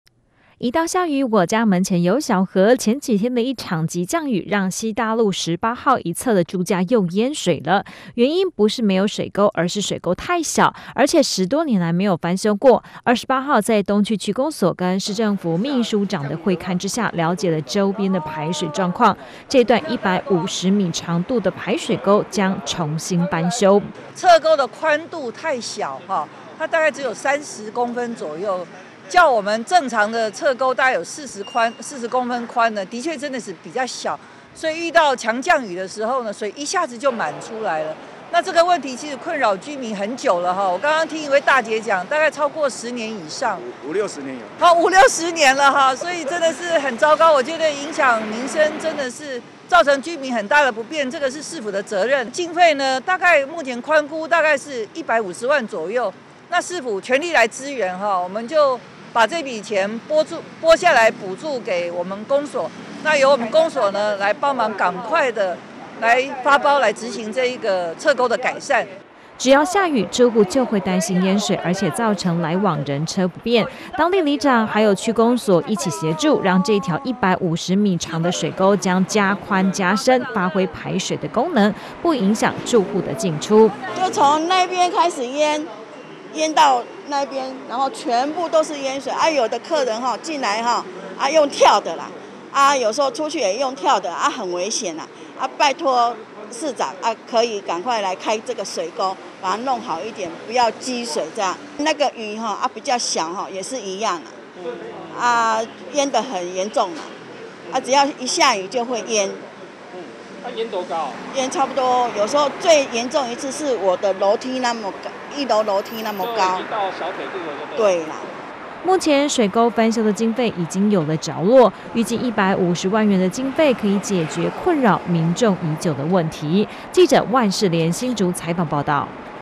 門前水溝一下雨就淹 現勘後市府允諾改善
前幾天的一場急降雨，除了讓水庫有進帳讓大家不用過度緊張，但是在西大路停七停車場對面的住家可就沒有這麼高興了。因為路邊水溝已有十多年沒有翻修加大，加上地勢較低，讓周邊住戶一遇到下雨就相當苦惱，28號在市府秘書長和東區區公所的會勘下，將要在汛期前完成側溝的整修工程。